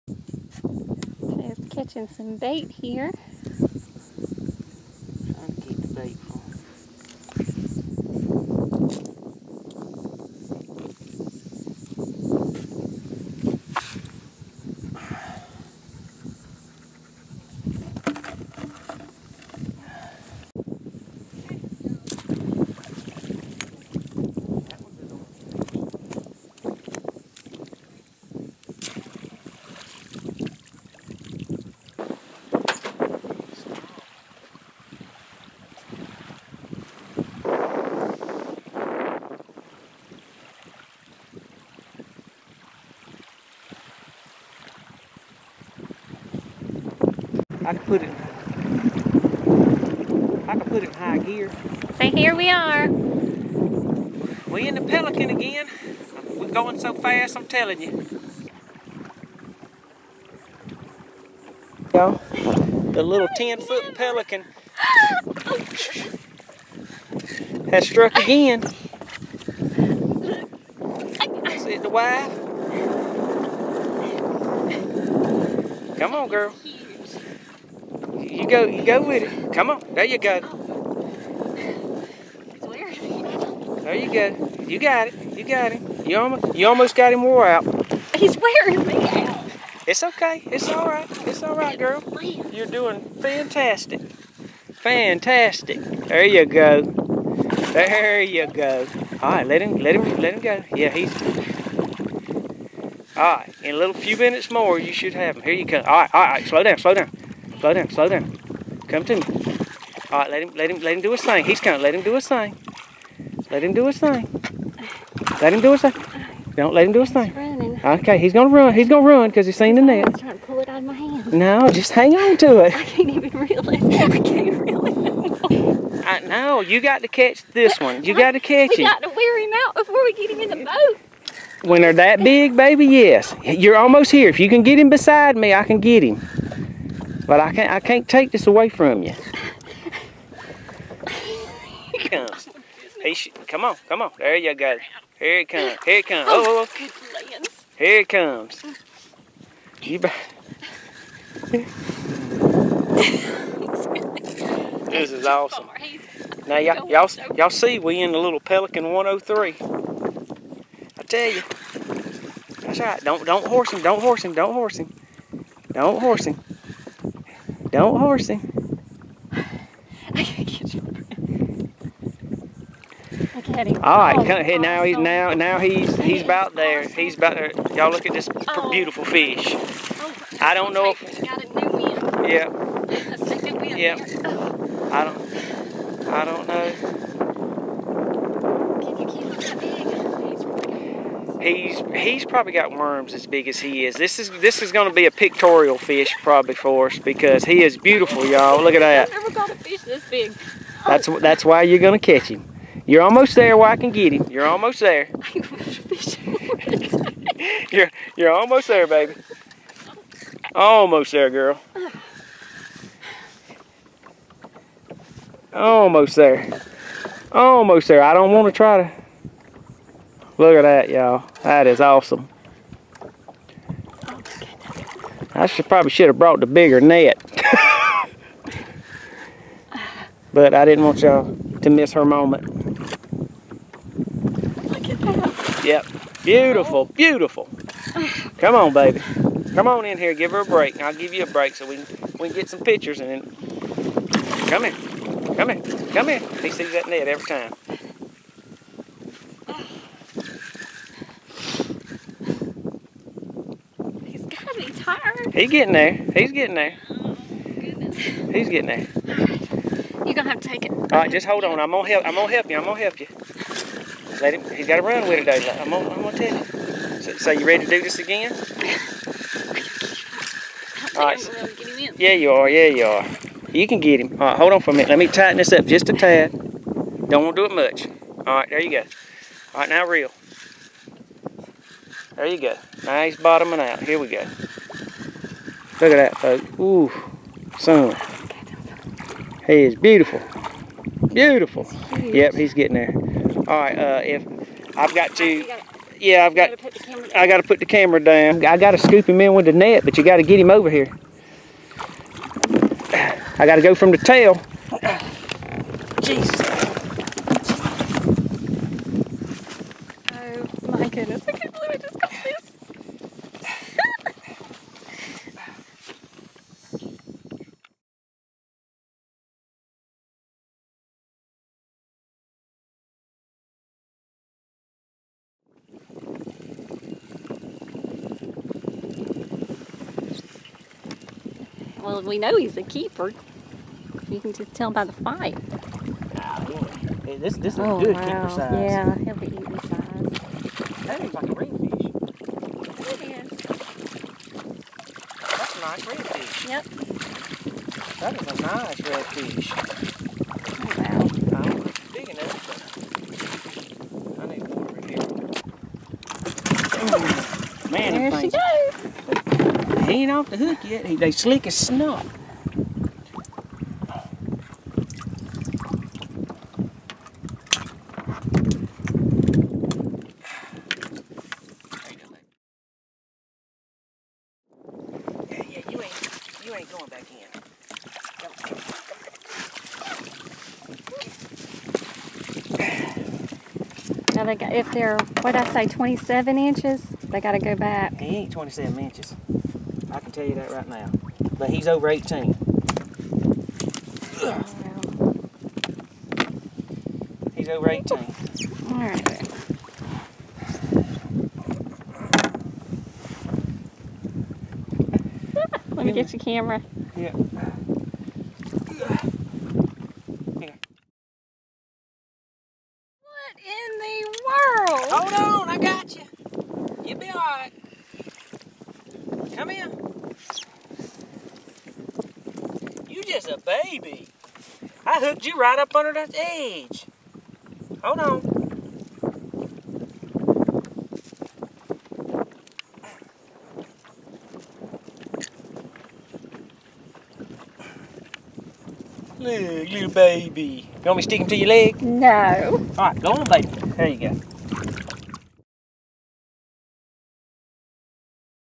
New Pelican Predator 103 Boat;Fishing at St. George Fl.;Biggest Fish my wife ever caught 40 lb drum
Hello,
My wife and I had a wonderful fishing trip at St. George Island , Fla., (Eastpoint).  We caught several redfish and drum.  However, my wife stole the show , she caught the fish of a life time!!!
Fishing Pelican Predator103#SaltwaterFishing